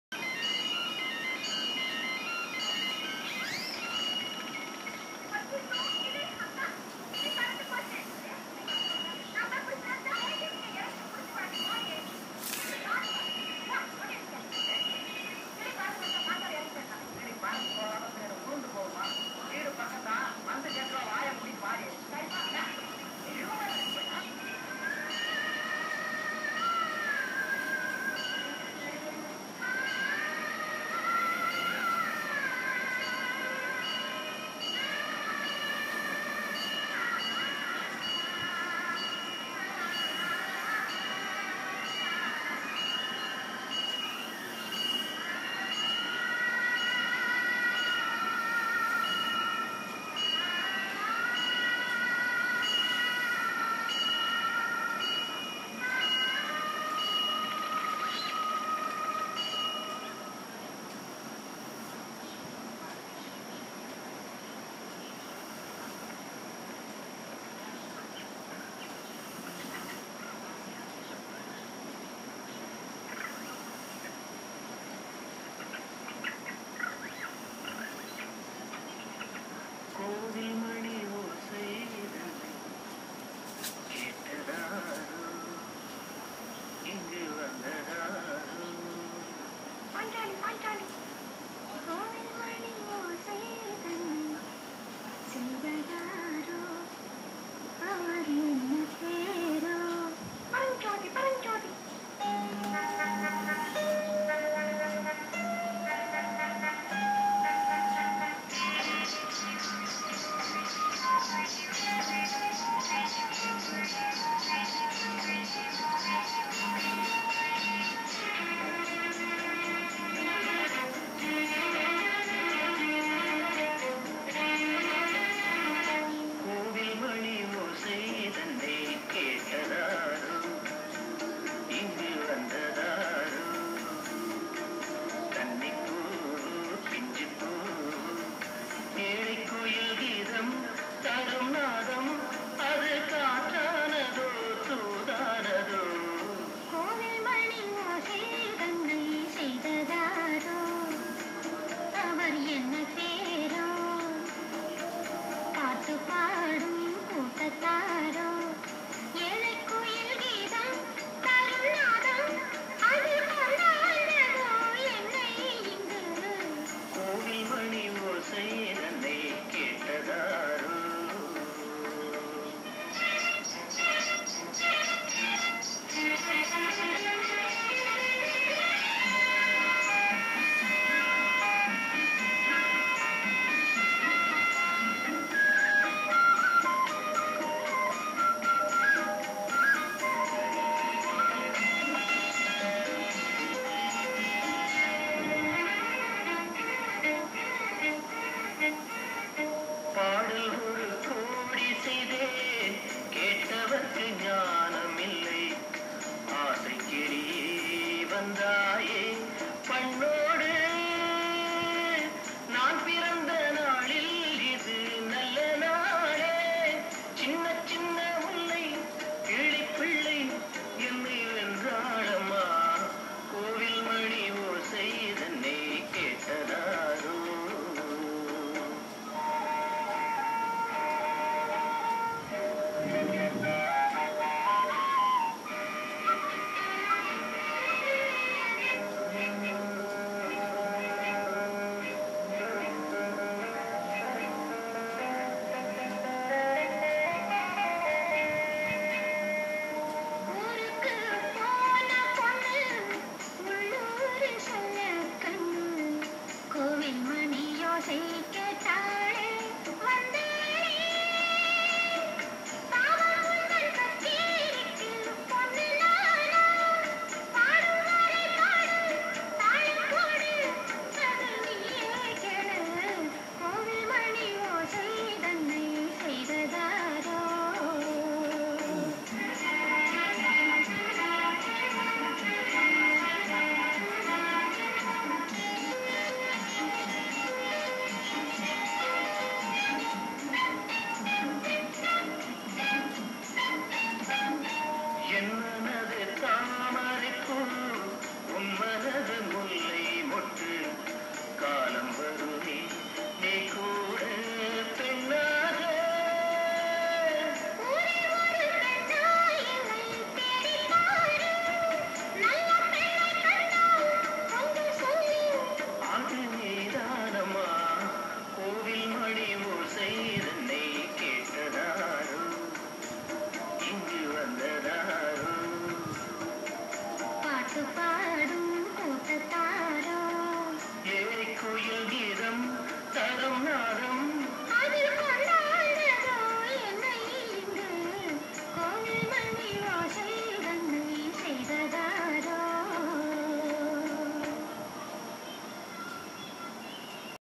Why the waterways run DRY now
All those days the river sand was not quarried allowing easy flow but now a days the pot holes are such in the river bed creating only cess pools but no Flow at all & that indicates that quarried sand went to erect multi story apartments & structures depriving the river it's' course for easy flow !
THAT's WHAT is called NEGATIVE DEVELOPEMENT !